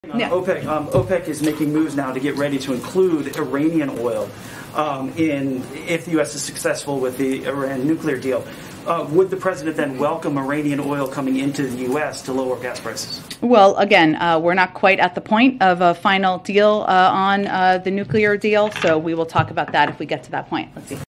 Asked If Biden Would "Welcome" Iranian Oil, Psaki Says "We'll Talk...If We Get To That Point"
When asked if President Joe Biden would “welcome” Iranian oil, President Joe Biden’s White House Press Secretary Jen Psaki said, “we’ll talk…if we get to that point,” during the White House press briefing on 2/25/2022.